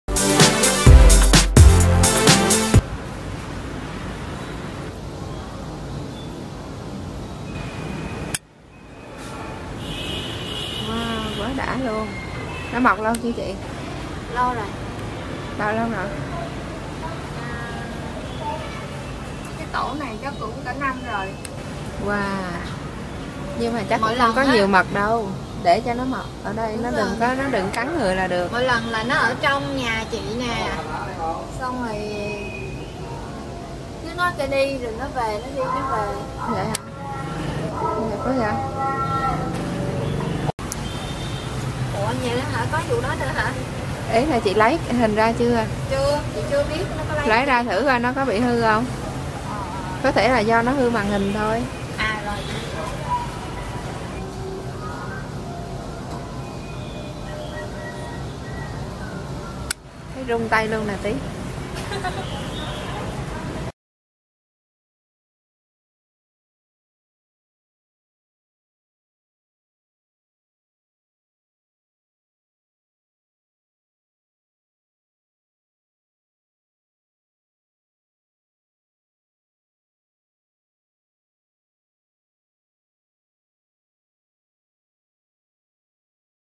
Nhiều điều kỳ lạ tại tổ ong khủng ở Sài Gòn
Tin báo mới hôm nay Độc đáo tổ ong khủng nhưng có nhiều nhiều kỳ lạ ở Sài Gòn.

Đây là tổ ong khủng nhất mà nó được hình thành trong một nhà dân tại TP.HCM.